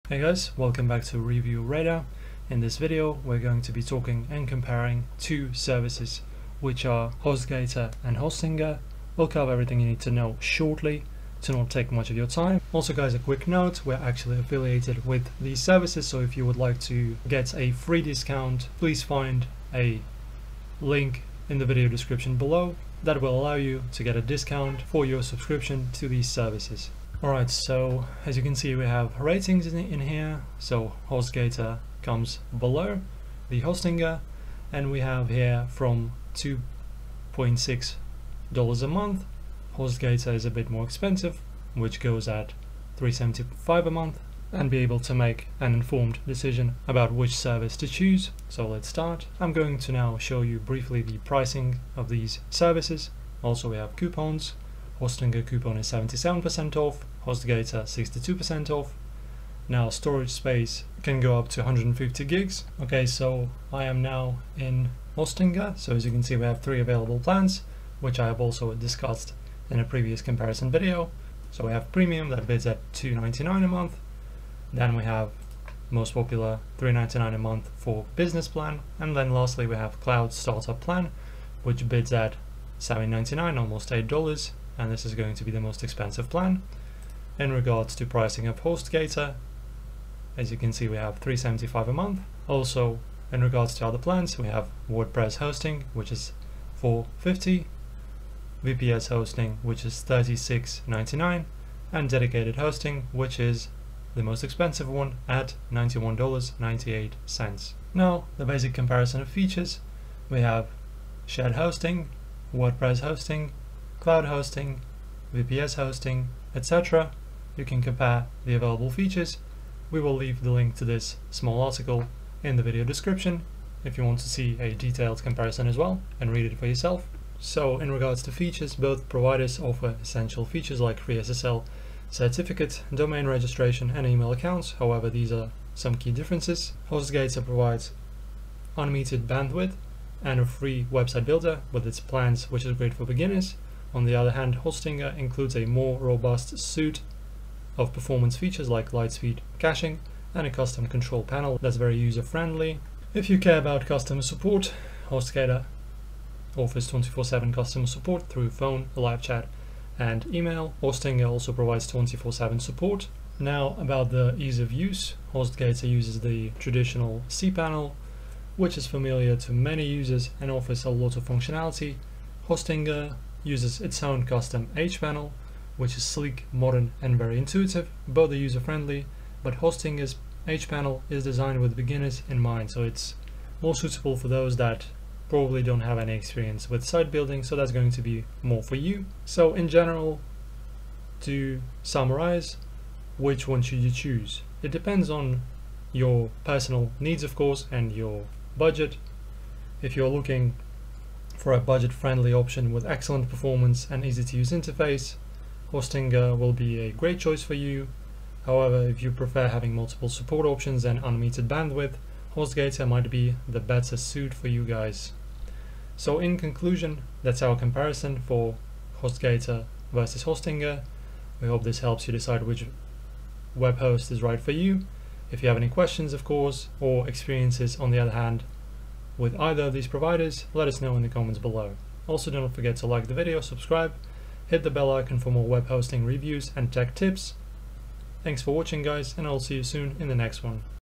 Hostinger vs Hostgator: Which is better? (2025)
In this video I compare the two webhosting services hostgator vs hostgator. After watching this video you will know which webhosting service is best for you.